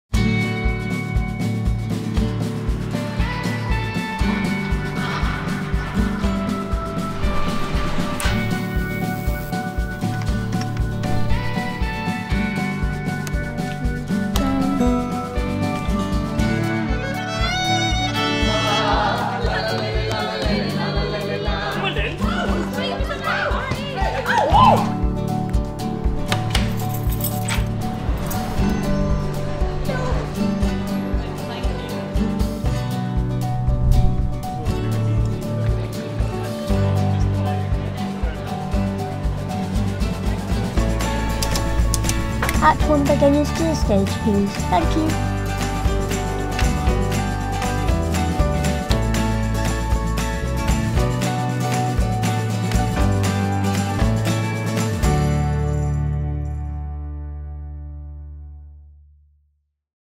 Behind the Curtain: A Love Letter to the King's
In June, we filmed this video during the first week of Sunshine on Leith, the last show in our programme at the theatre before it closes for a Capital Redevlopment Project